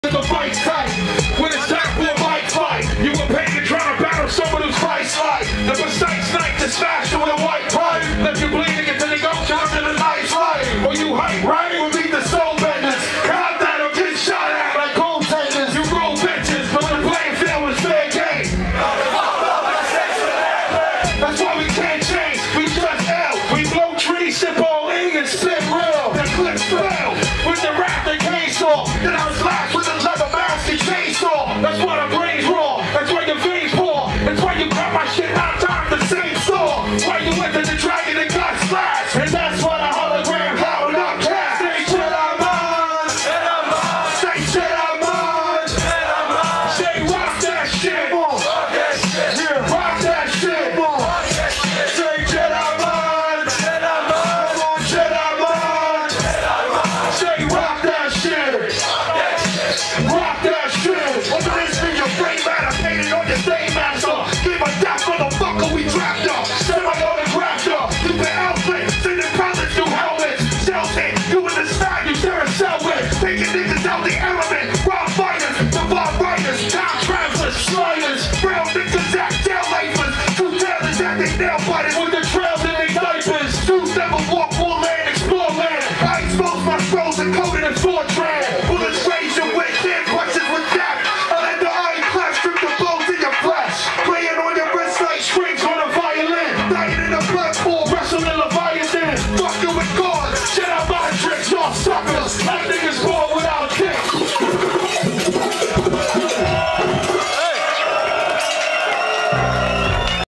Jedi Mind Tricks Melkweg
show from Jedi Mind Tricks in HQ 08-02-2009 in De Melkweg, Amsterdam, The Netherlands